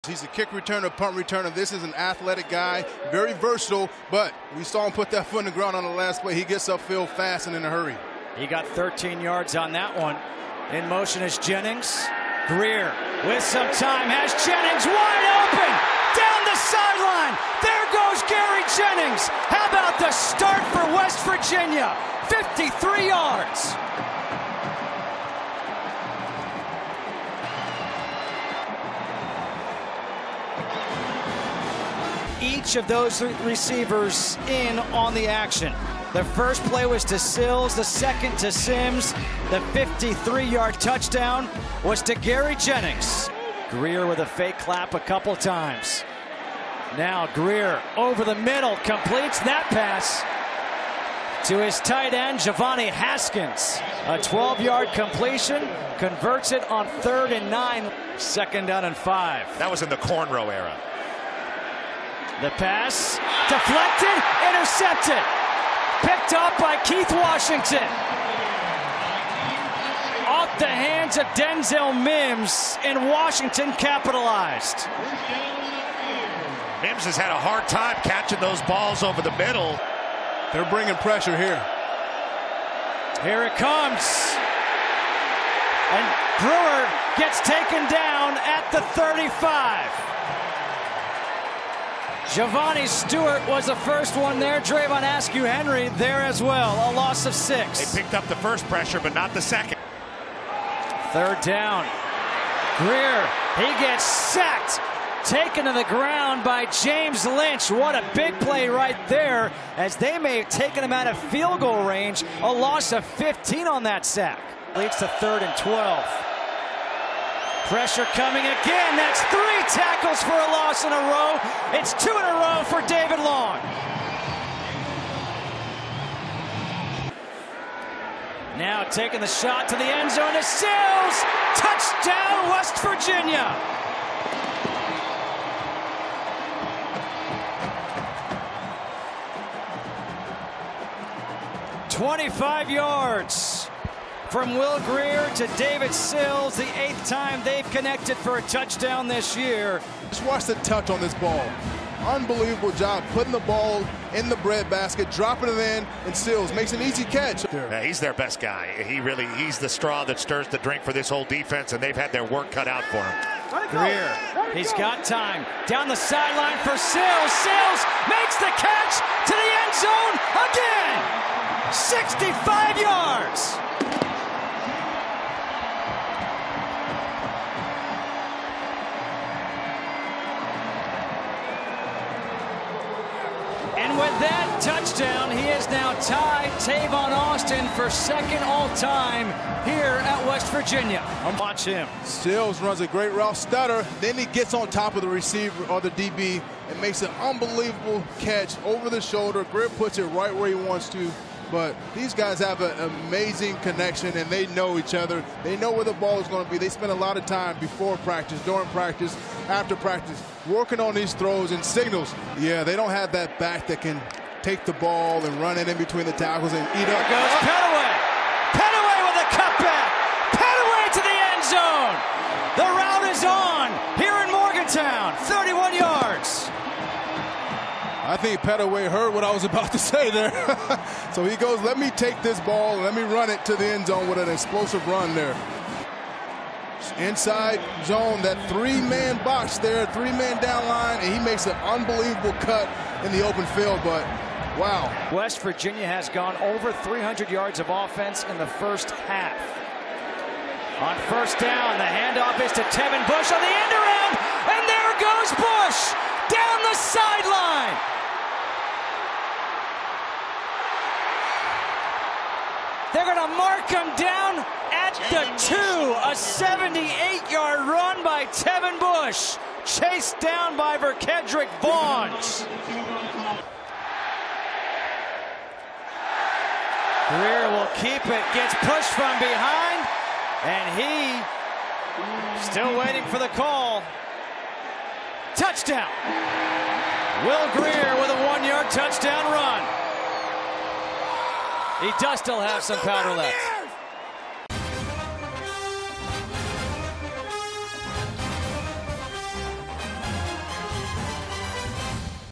West Virginia vs. Baylor | FOX COLLEGE FOOTBALL HIGHLIGHTS
No. 13 West Virginia roll Baylor in convincing fashion. The Mountaineers racked up over 550 yards of total offense. WVU improves to 6-1 on the season.